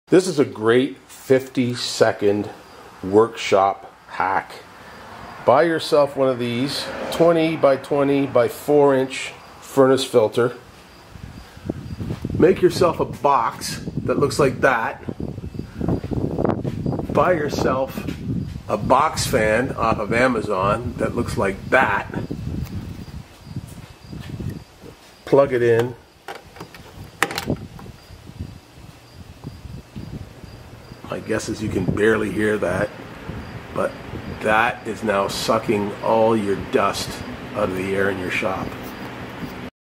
Mr. Knuckle's Workshop - A (Relatively) Cheap way of Getting Dust Out of the Air in Your Workshop
Dust is the bane of the woodworker's existence. Get rid of this health hazard with this fairly cheap and easy-to-make airborne dust extractor.